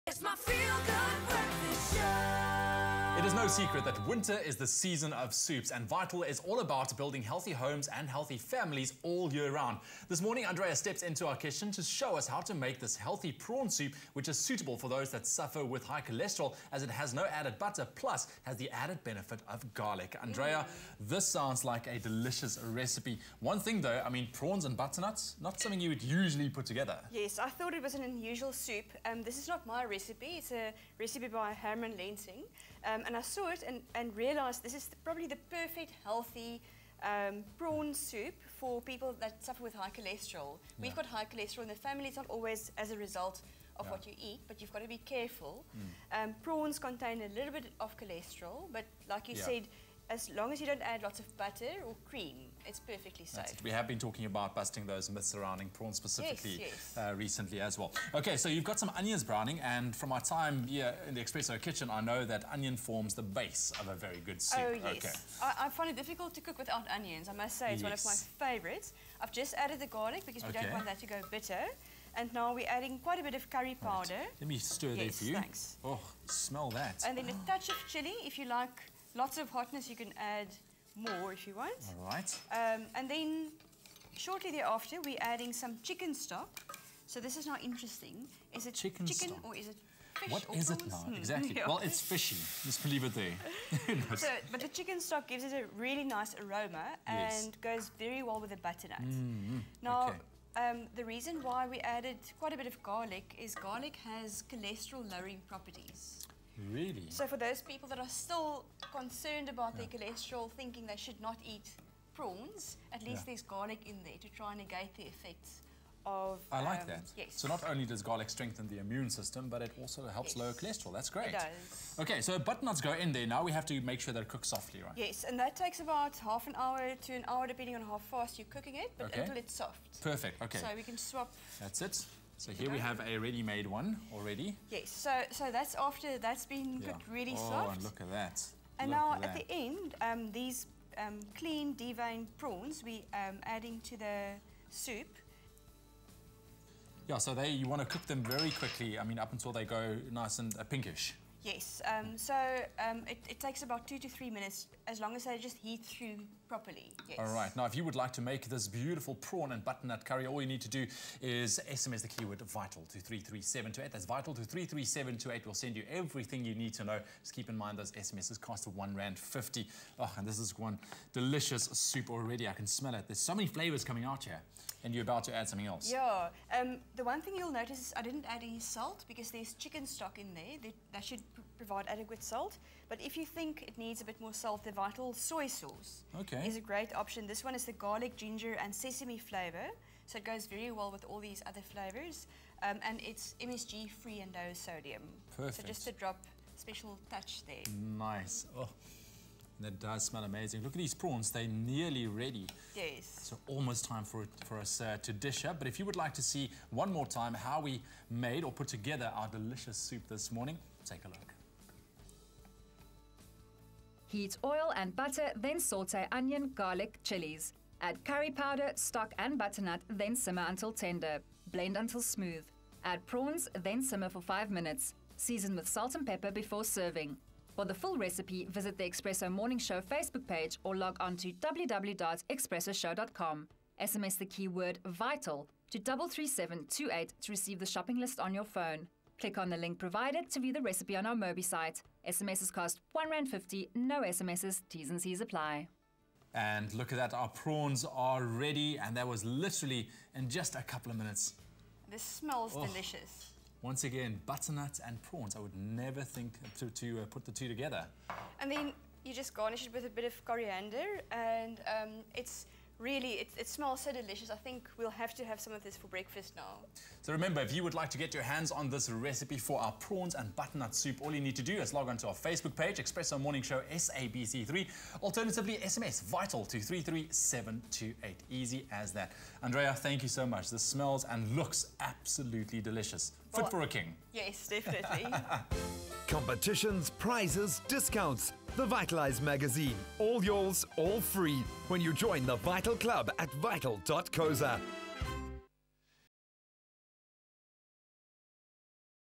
VITAL - Butternut and Prawn Soup (20 August 2014)
Winter is the season of soups, and Vital is all about building healthy homes and healthy families all year around! Andrea is here to show us how to make this  healthy prawn soup, which is suitable for those that suffer with high cholesterol as it has no added butter, plus has to added benefit of garlic!